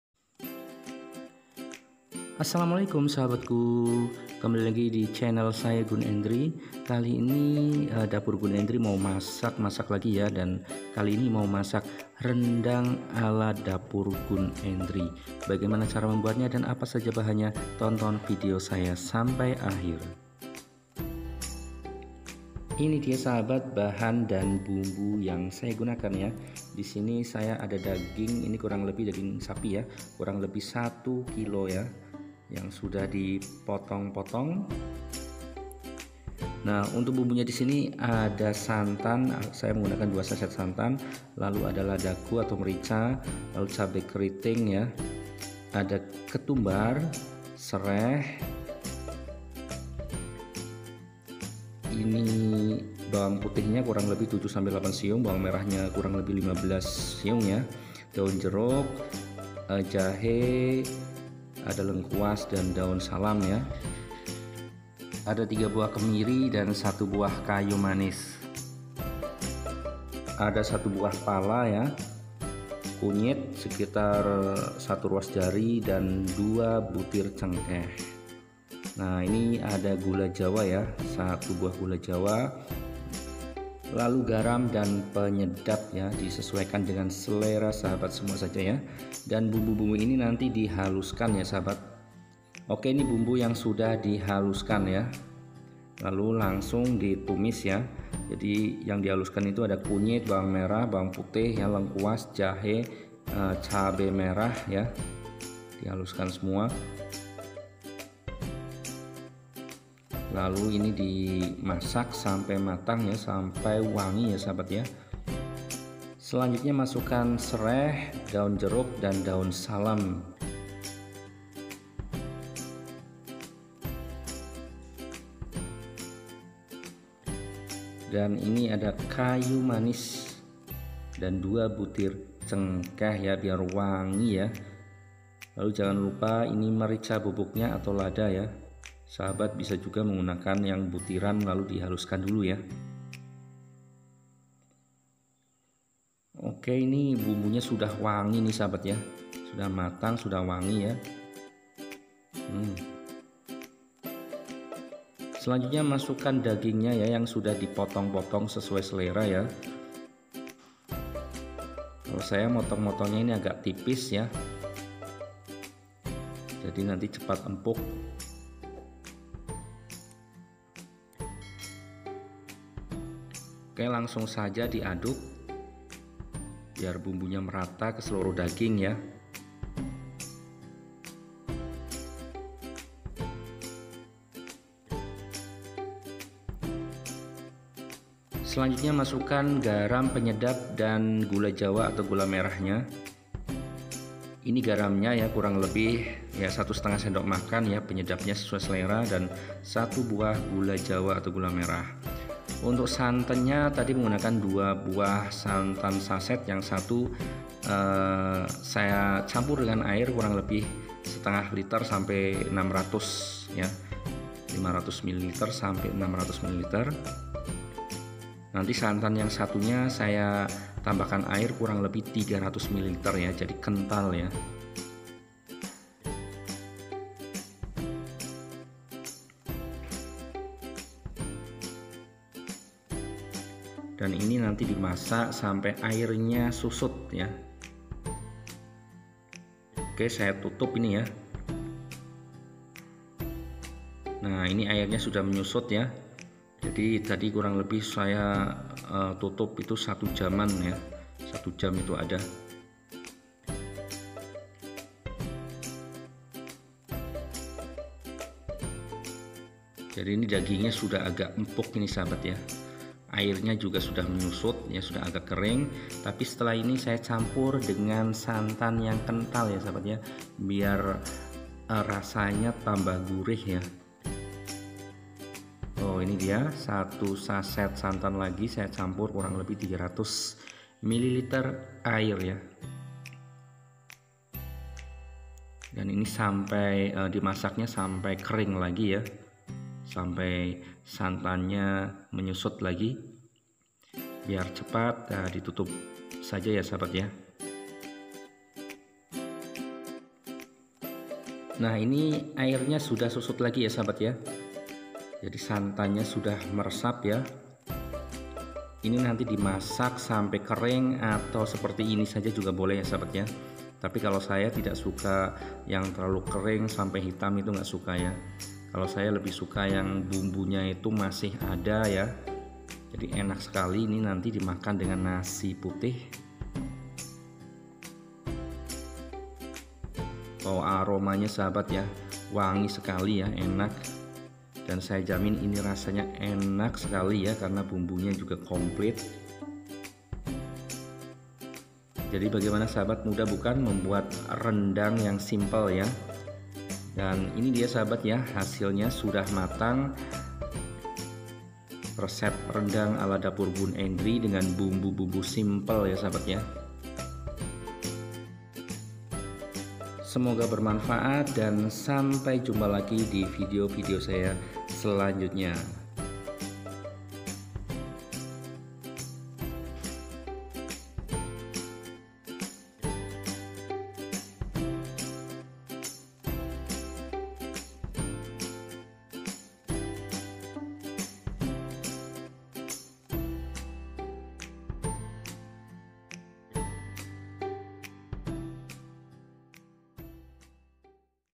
Rendang Daging Sapi, Sederhana bikin ketagihan
Bahan & Bumbu :
1. Daging sapi 1 kg
2. santan kara 2 sachet
3. Bawang merah 15 siung
4. Bawang putih 8 siung
5. 1 SDM ketumbar
6. merica
7. kunyit 1 ruas jari
8. Jahe
9. Lengkuas
10 Kemiri
11. Sereh, daun salam, daun jeruk
12. pala 1 buah
13. kayu manis
14. cengkeh
16. garam 1½ SDM
17. gula Jawa 1 buah
18. penyedap
19. air ± 800 ml